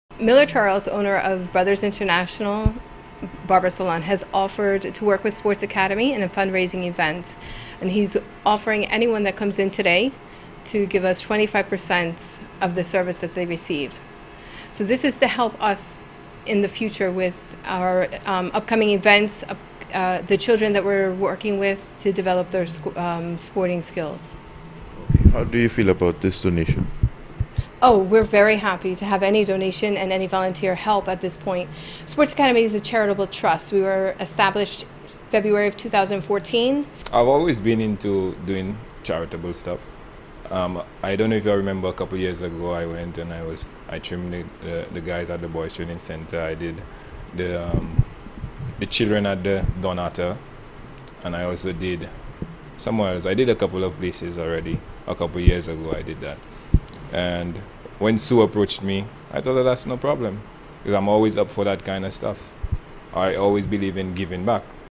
salon clip
Description ENDS

The Brothers International Barber Salon is  providing financial support to the Sports Academy to help the newly formed organization with it’s sporting activities.

The Salon devoted today to raising funds for the academy.

Twenty five percent of the day’s sales went to the  new organization.

It’s been only nine months since the Accademy opened it’s doors

 Despite that  it  says it has already made some significant strides.

In July the Accademy organized a five week summer camp.

The Academy has also  been able to establish a swim team that has already competed at  the OECS level.